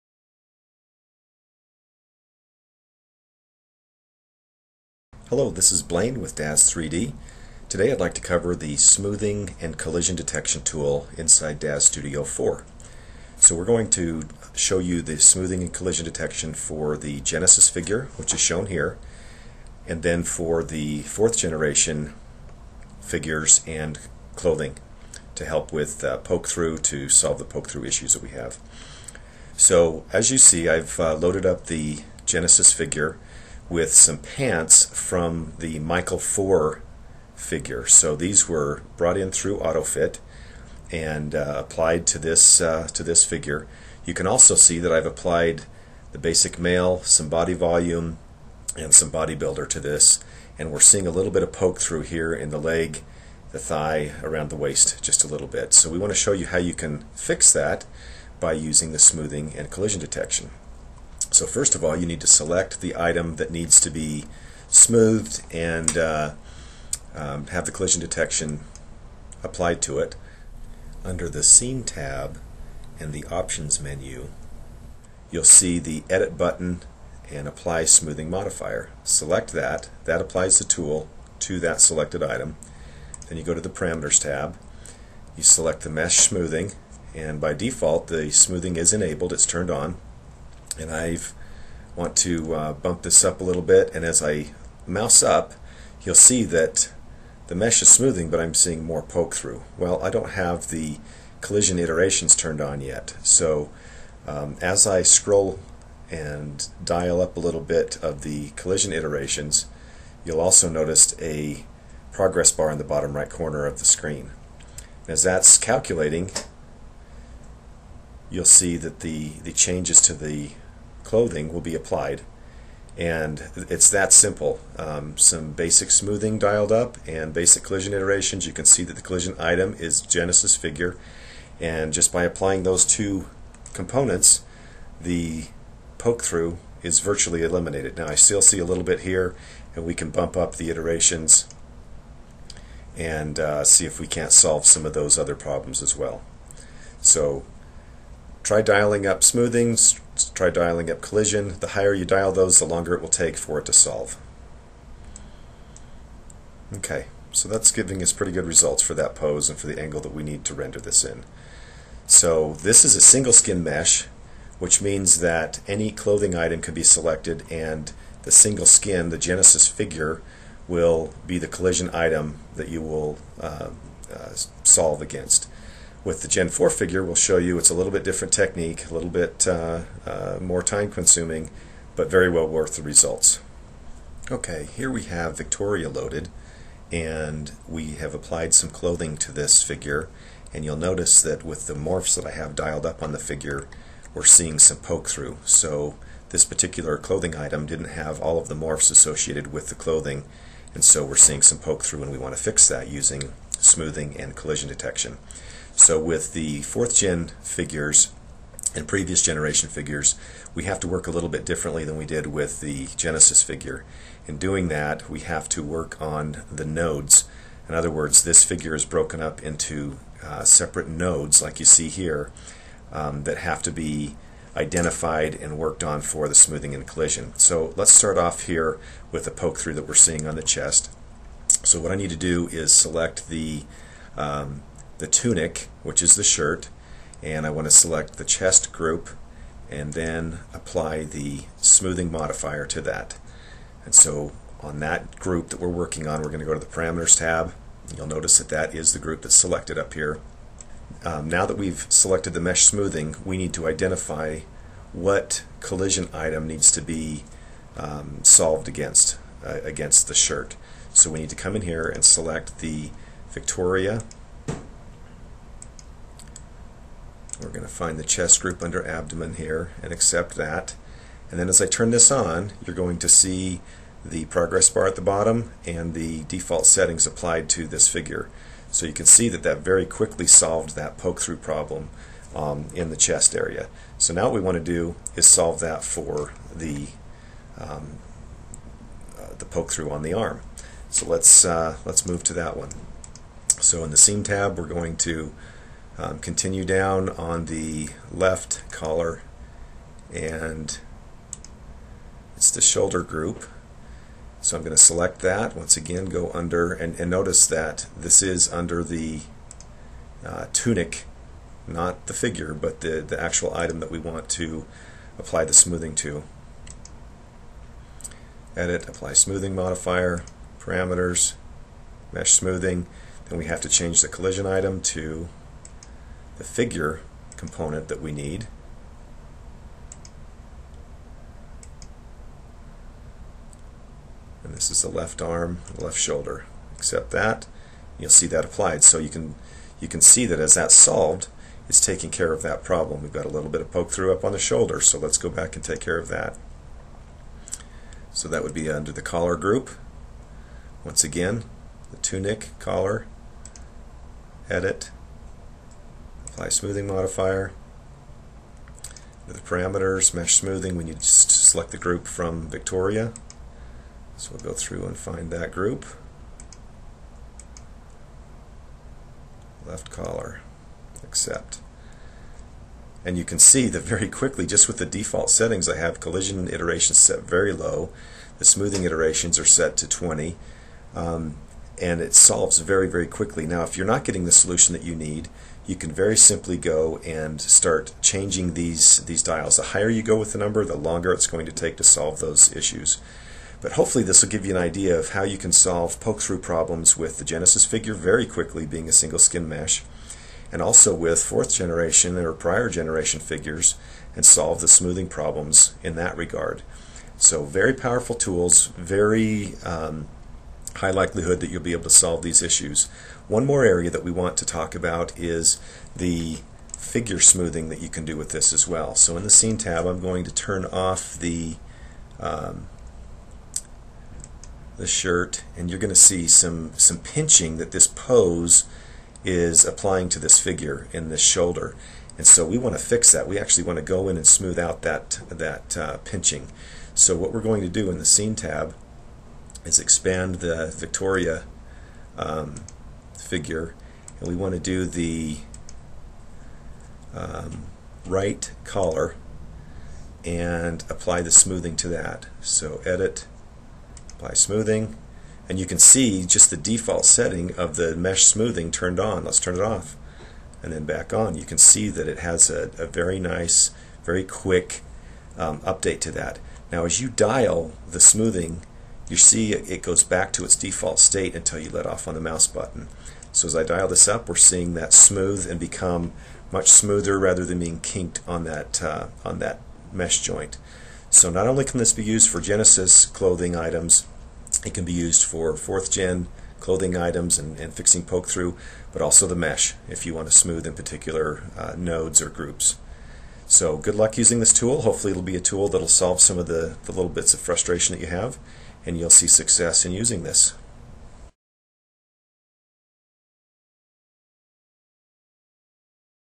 DAZ Studio 4 - Smoothing and Collision Detection Genesis and Gen 4
DAZ Studio 4.0 has a new tool for fixing poke through issues on any figure. We will show how to fix issues with the Genesis figure, Victoria 4 clothing and smooth a joint on Victoria's shoulder.

If you are frustrated with poke-through, this is a must see training video.